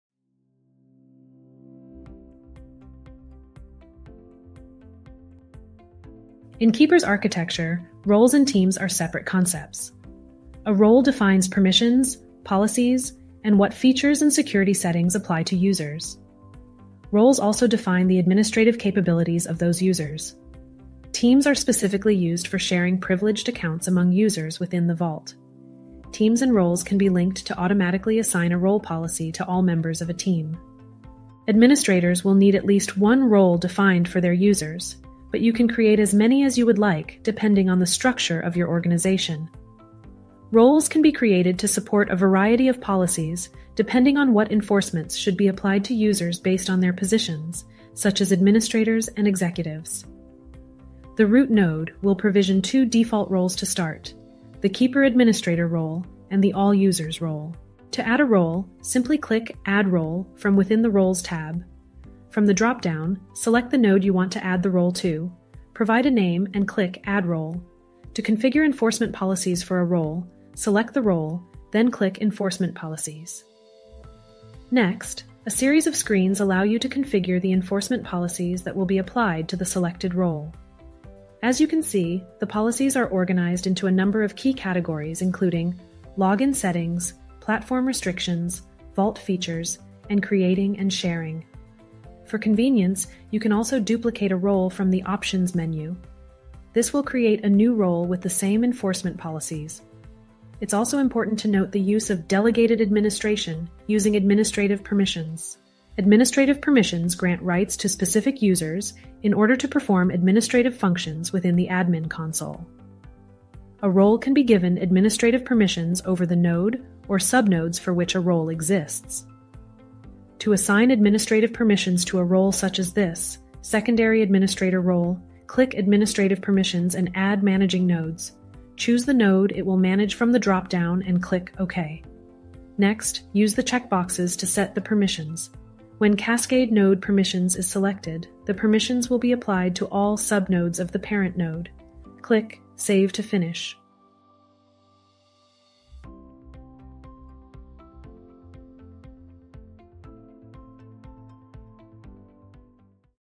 Keeper 101 | Enterprise - How to Set Role-based Access and Controls in Keeper
Learn how to set role-based access and controls in Keeper with our step-by-step guide.

Transcript

In Keeper’s architecture, Roles and Teams are separate concepts. A Role defines permissions, policies and what features and security settings apply to users. Roles also define the administrative capabilities of those users. Teams are specifically used for sharing privileged accounts among users within the vault. Teams and Roles can be linked to automatically assign a role policy to all members of a team.

Administrators will need at least one role defined for their users, but you can create as many as you would like depending on the structure of your organization. Roles can be created to support a variety of policies depending on what enforcements should be applied to users based on their positions, such as administrators and executives.

The root node will provision two default roles to start, the “Keeper Administrator” role and the “All Users” role.

To add a role, simply click Add Role from within the Roles tab. From the dropdown, select the node you want to add the role to; provide a name and click Add Role. To configure enforcement policies for a role, select the role, then click Enforcement Policies. Next, a series of screens allow you to configure the Enforcement Policies that will be applied to the selected role. As you can see, the policies are organized into a number of key categories including: Log in Settings, Platform Restrictions, Vault Features, and Creating and Sharing. For convenience, you can also duplicate a role from the “Options” menu. This will create a new role with the same enforcement policies.

It’s also important to note the use of “delegated administration’” using Administrative Permissions. Administrative Permissions grant rights to specific users in order to perform administrative functions within the admin console. A role can be given Administrative permissions over the node or sub-nodes for which a role exists.

To assign administrative permissions to a role such as this, Secondary Administrator role, click Administrative Permissions and Add Managing Notes. Choose the node it will manage from the dropdown and click OK. Next, use the checkboxes to set the permissions. When “Cascade Node Permissions" is selected, the permissions will be applied to all sub-nodes of the parent node. Click Save to finish.